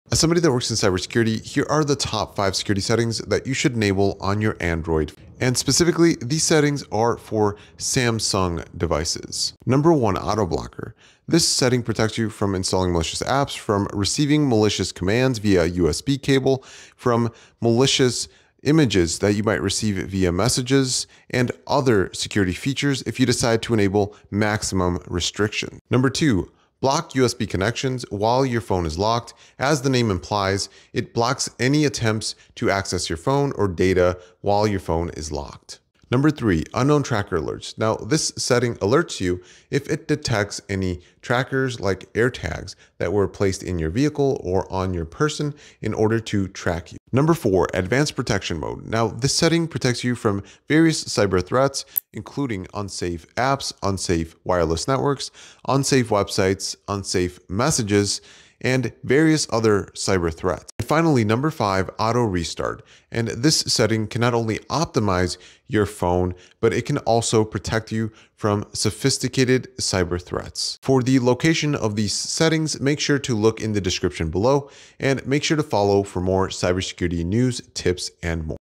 Top 5 Security Settings to Enable On Android
1. Auto Blocker: Enable this setting to block the installation of malicious apps, malicious commands sent when connected via USB, malicious images in your messages and more. Consider enabling maximum restrictions for max security. Setting found here:

Security & Privacy - Auto Blocker

2. Block USB connections while locked: This settings prevents any USB data transfer while your phone is locked for more than 2 hours. Protecting you in the event you lose your phone or leave it alone for an extended period.

Security & Privacy - More Security Settings - Block USB connections while locked.

3. Unknown Tracker Alerts: This setting protects you by alerting you of any trackers like air tags that might be in your vehicle or person. Setting found here:

Google - Google Services - Unknown Trackers

4. Advanced Protection Mode: Protects you from advanced cyber threats including unsafe apps, websites, WiFi networks and more. Setting found here:

Google - Google Services - Advanced Protection Mode

5. Auto Restart: This setting allows you to schedule restarts of your phone and allow your phone to restart when it detects an issue. Restarting regularly not only optimizes your phone, but also protects you from advanced cyber threats like spyware. Setting found here:

Device Care - Auto Optimization - Auto Restart